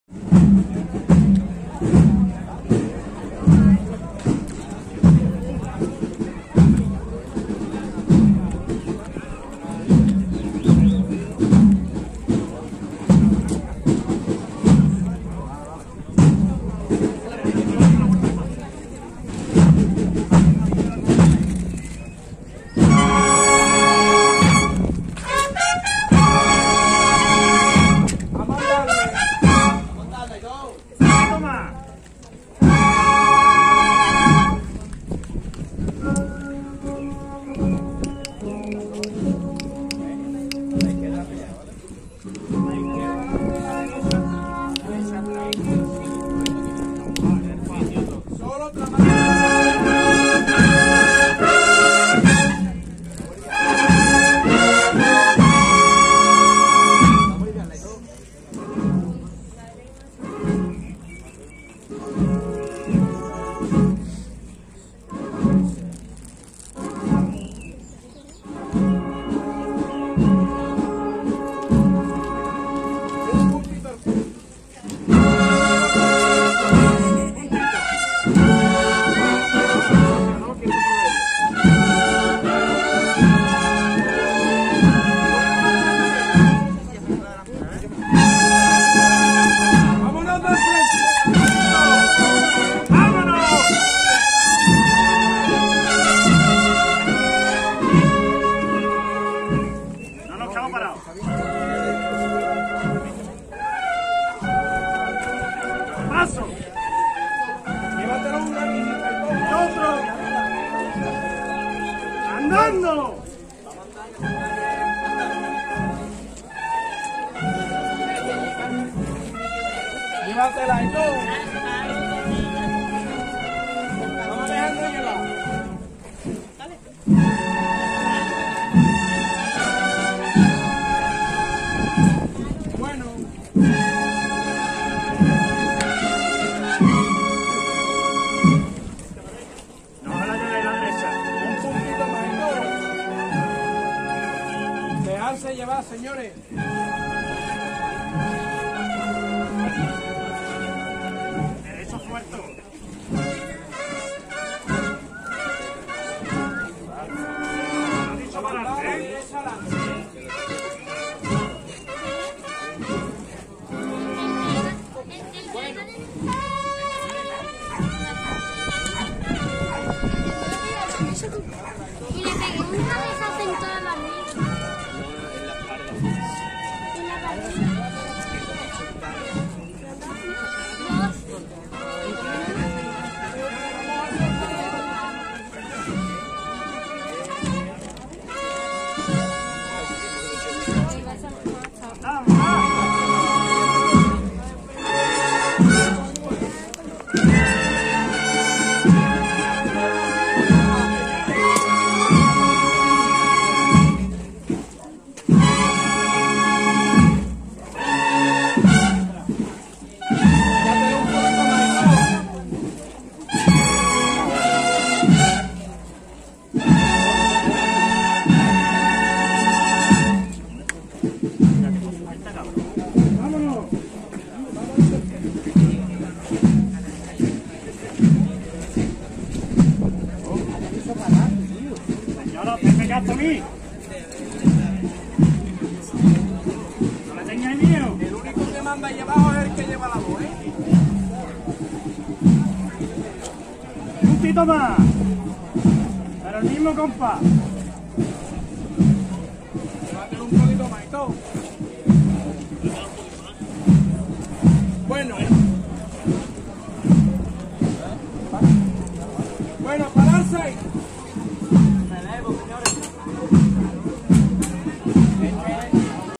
4K || CRUZ DE MAYO "LOS MADRILEÑOS" | CARIDAD | BARRIADA DE LOS MADRILEÑOS | EL PUERTO "CÁDIZ" 2024
📍 BARRIADA DE LOS MADRILEÑOS "EL PUERTO DE SANTA MARÍA" CÁDIZ
🎺 BANDA DE CORNETAS Y TAMBORES "CARIDAD" JEREZ DE LA FRONTERA
🎥 POLILLA_COFRADE